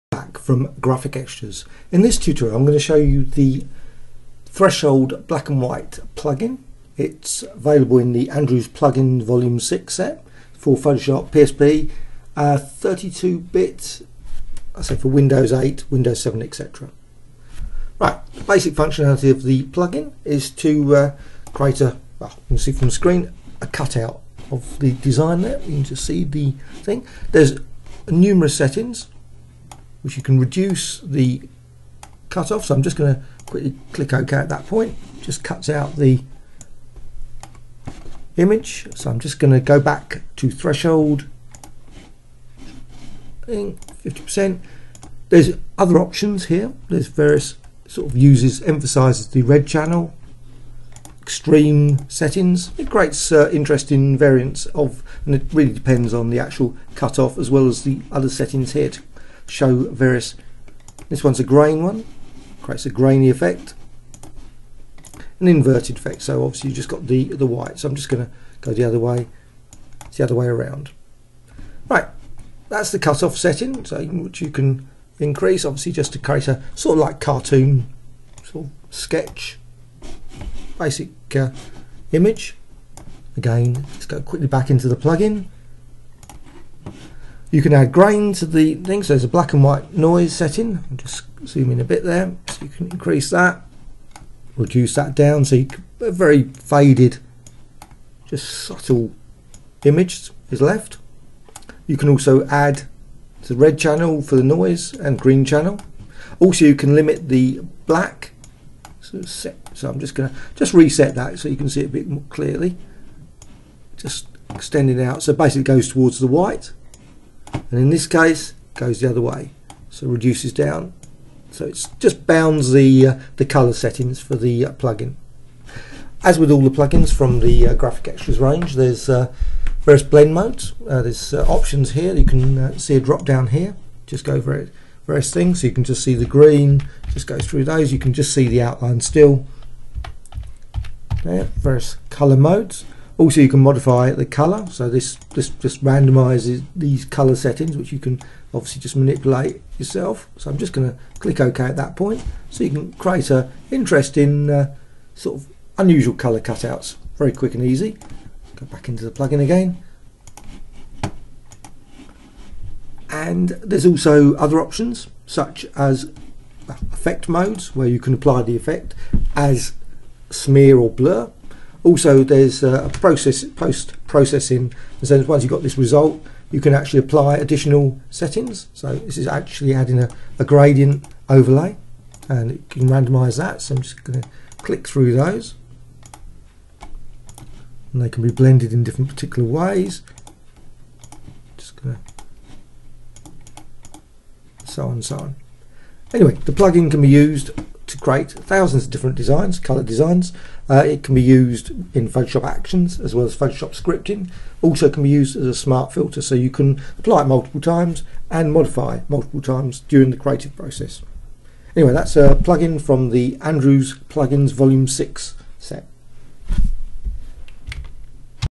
Photoshop Plugins : How To Cut Out Images Plugin Tutorial | Graphicxtras
A tutorial showing the threshold BW / cutout image plug-in for use in Photoshop and PSP PC (Andrew's plugins 6)